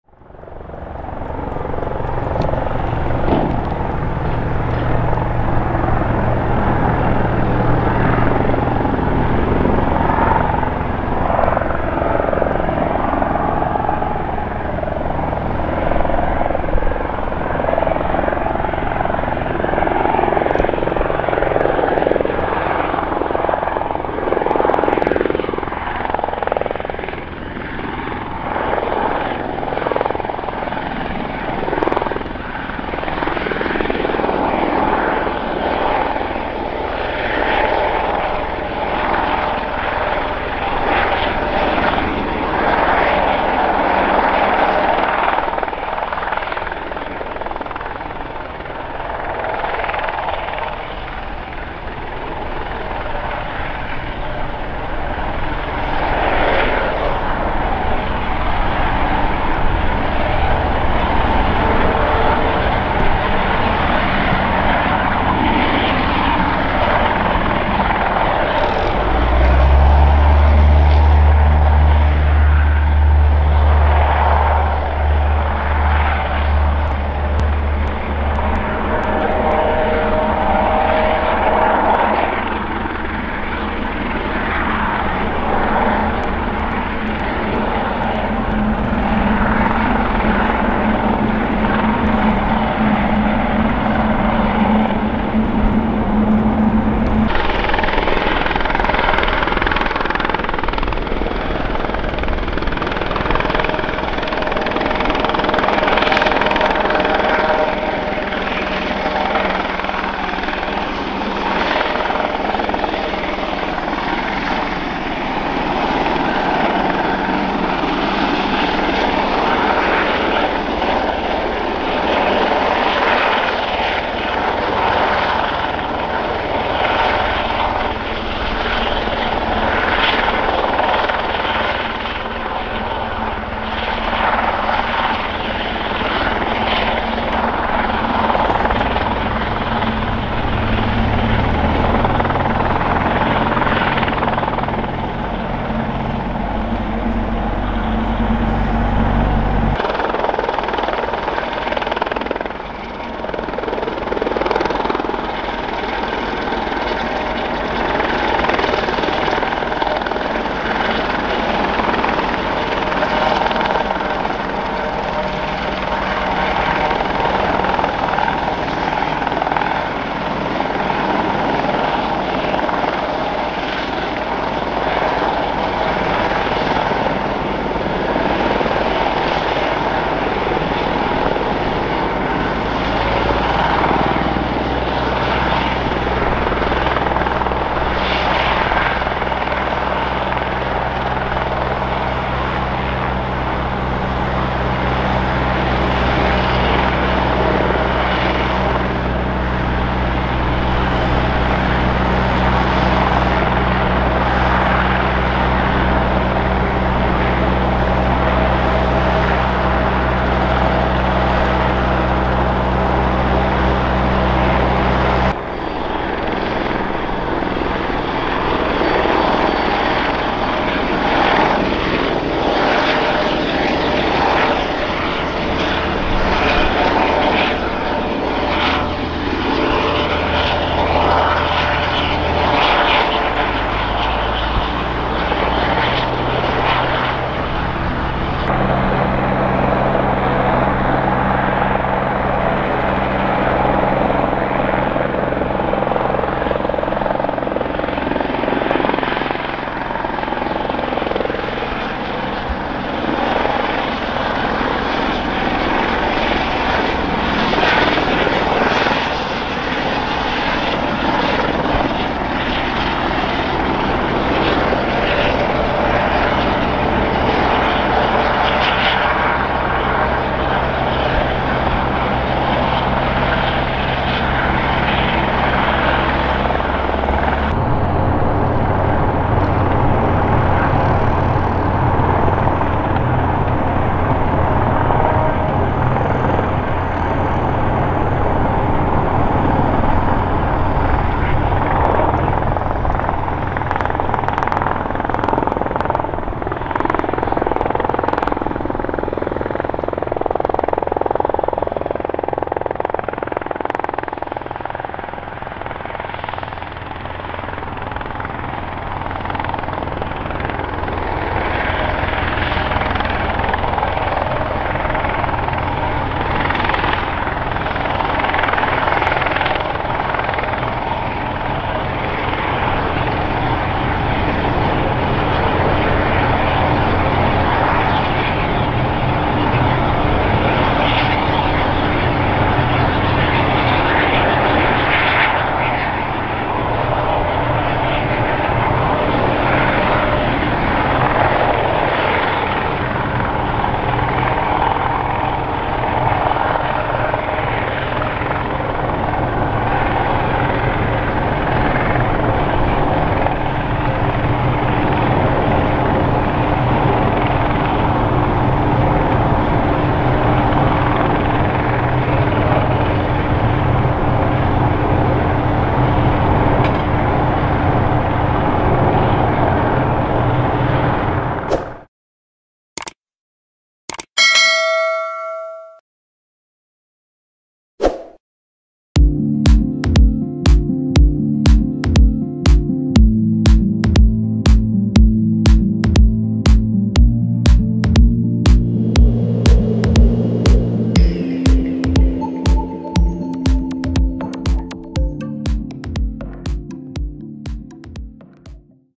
Bonn-Hangelar Airport (EDKB) - Airbus EC120B Colibri Bundespolizei Hubschrauber Helicopter
Helicopter fleet of the federal police returns to their base after an exercise.

► Summary of this video:
00:00 Eurocopter EC120B Bundespolizei
06:10 Subscribe Dotwood Media - Thank you!

Dotwood Media 🔜🎥